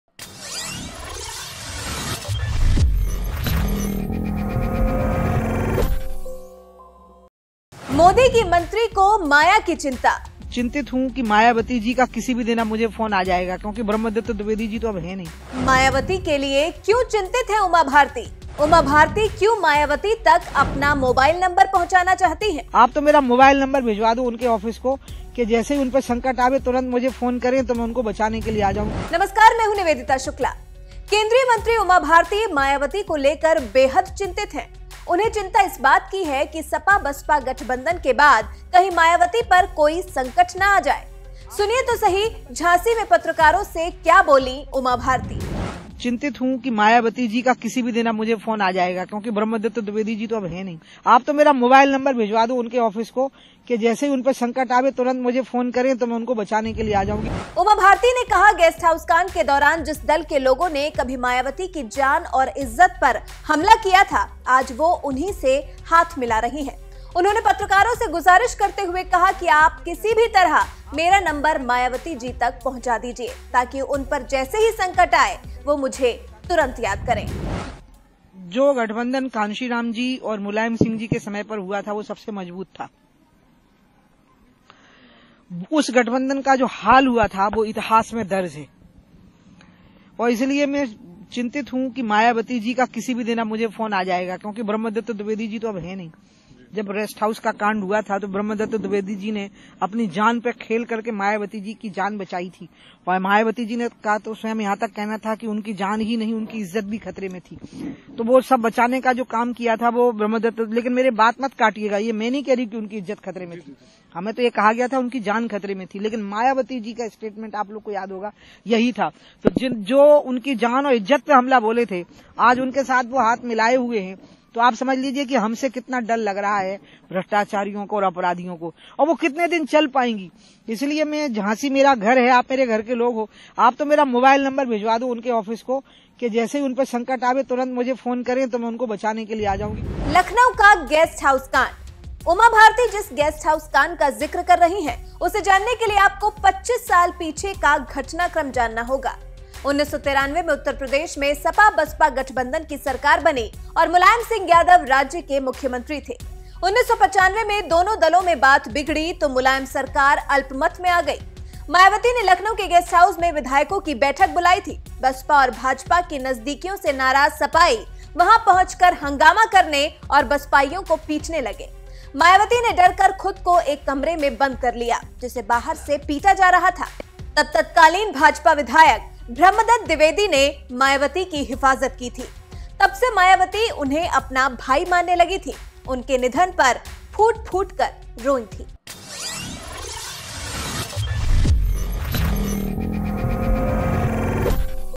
Uma Bharti को सता रही Mayawati की चिंता, आखिर किससे है Maya को खतरा ?
उमा भारती को अचानक बसपा सुप्रीमो मायावती की चिंता सताने लगी है और उन्होंने पत्रकारों से गुजारिश की है कि वो किसी भी तरह मायावती तक उनका मोबाइल नंबर पहुंचा दें...